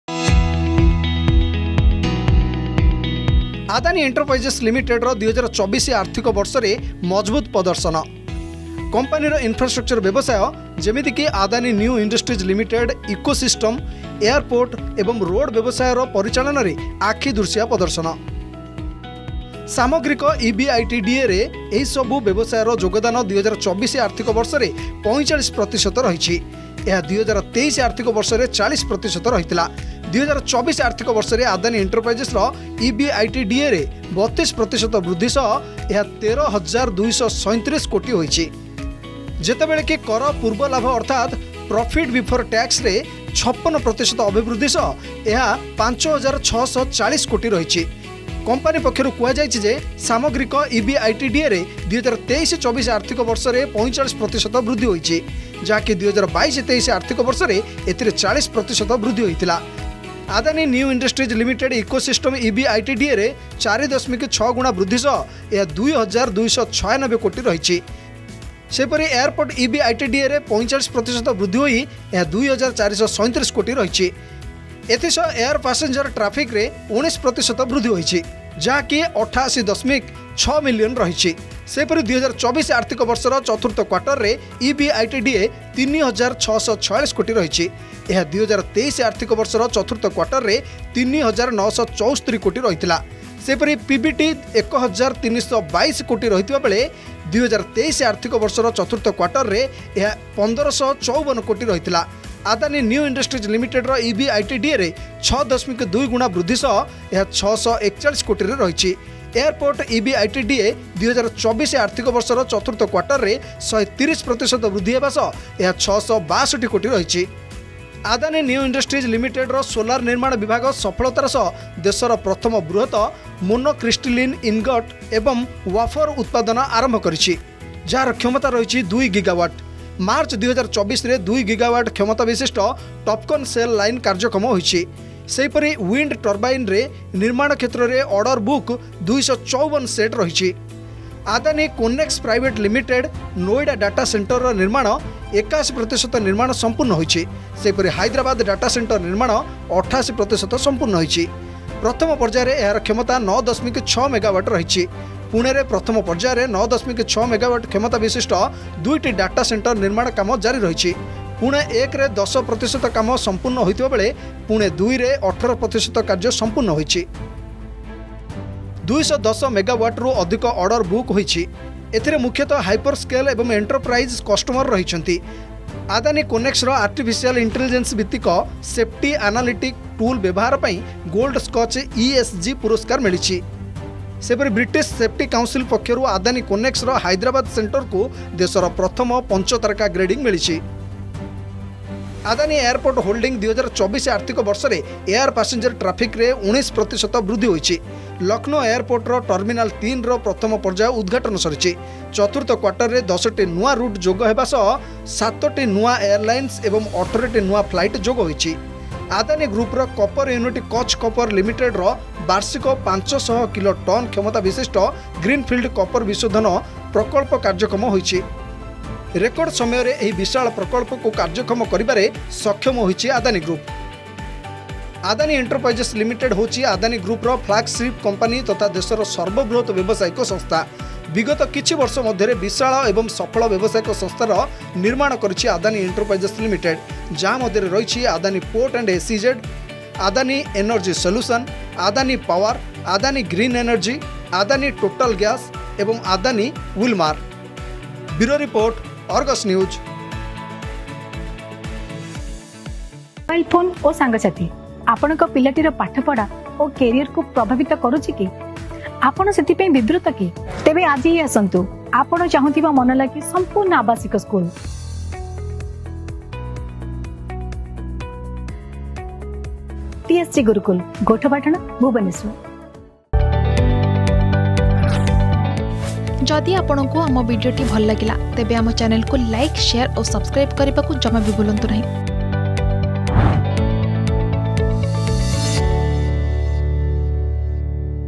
ଆଦାନୀ ଏଣ୍ଟରପ୍ରାଇଜର୍ସ ଲିମିଟେଡର ୨୦୨୪ ଆର୍ଥିକ ବର୍ଷରେ ମଜଭୂତ ପ୍ରଦର୍ଶନ || Adani Group
ଆଦାନୀ ଏଣ୍ଟରପ୍ରାଇଜର୍ସ ଲିମିଟେଡର ୨୦୨୪ ଆର୍ଥିକ ବର୍ଷରେ ମଜଭୂତ ପ୍ରଦର୍ଶନ। କମ୍ପାନୀର ଇନଫ୍ରାଷ୍ଟ୍ରକ୍ଚର ବ୍ୟବସାୟ ଯେମିତି ଆଦାନୀ ନ୍ୟୁ ଇଣ୍ଡଷ୍ଟ୍ରୀଜ ଲିମିଟେଡ୍ ଇକୋସିଷ୍ଟମ, ଏୟାରପୋର୍ଟ ଏବଂ ରୋଡ୍ ବ୍ୟବସାୟର ପରିଚାଳନାରେ ଆଖିଦୃଶିଆ ପ୍ରଦର୍ଶନ।